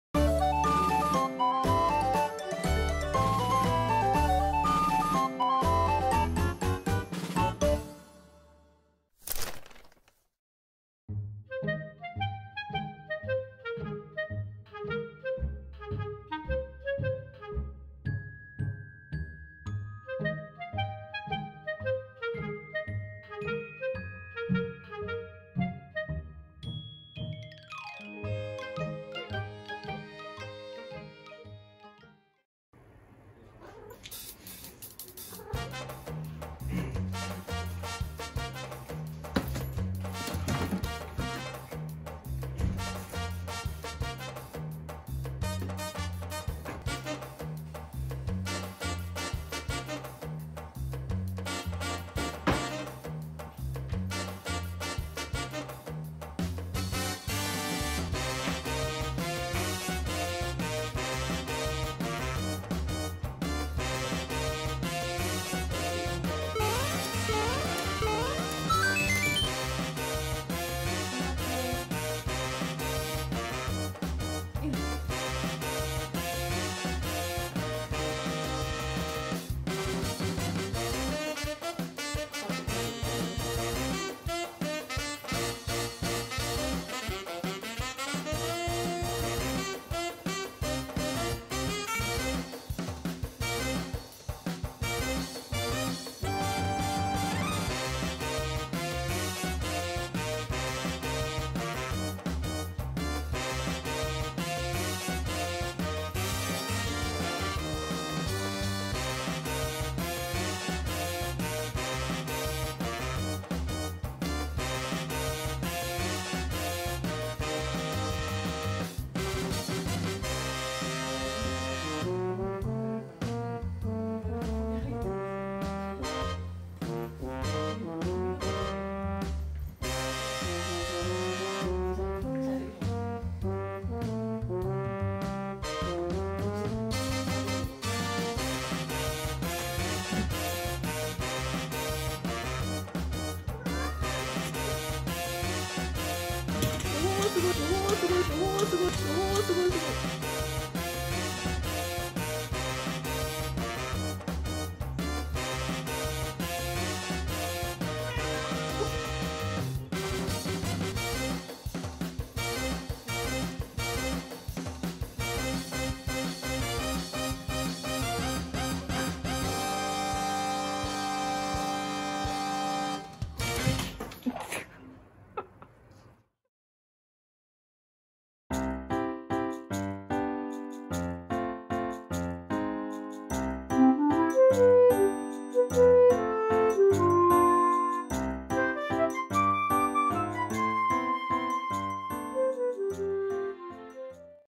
【捕獲】ちょっと届かないかもしれない【保護猫】
トカゲのマグネットを見た途端に野生化したぽん太くん。
野良時代に食べていたのでしょうか…